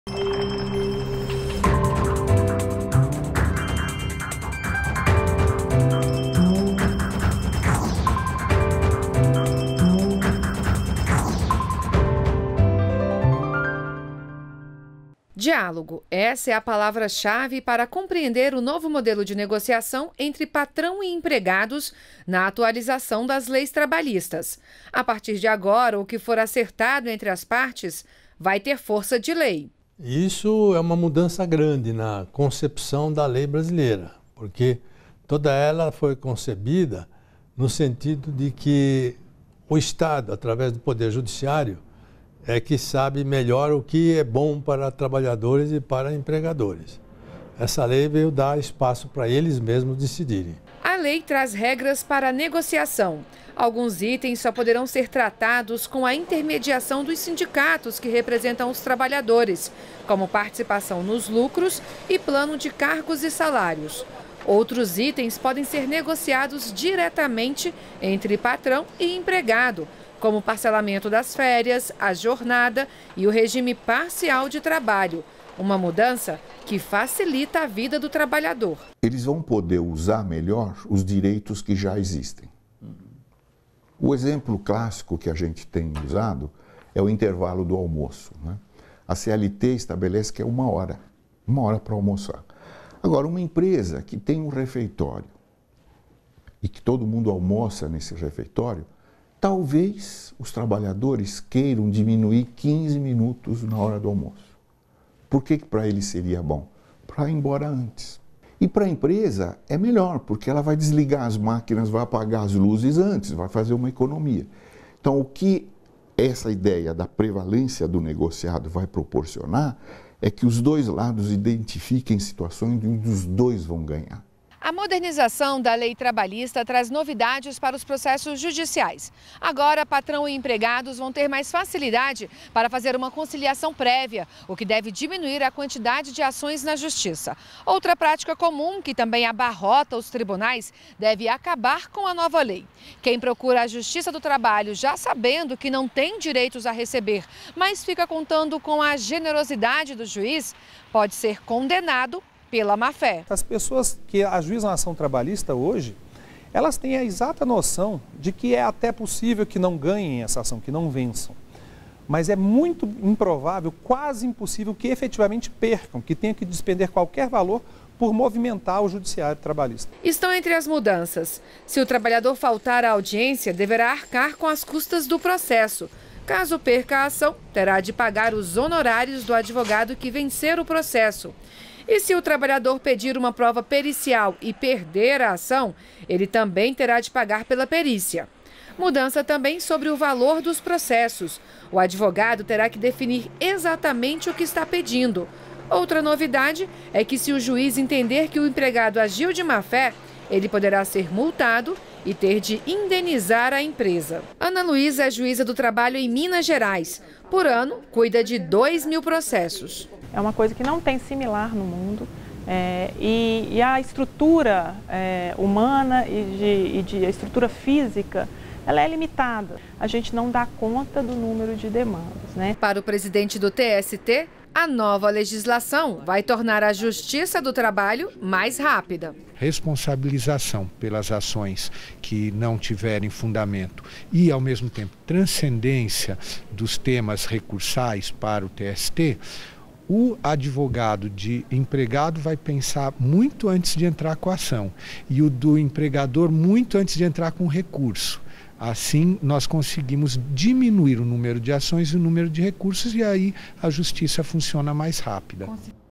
Nova lei trabalhista favorece o diálogo entre empregado e empregador
REPÓRTER NBR - 07.11.17: O acordado entre patrão e empregado tem força de lei com a modernização trabalhista e o número de processos na justiça deve diminuir.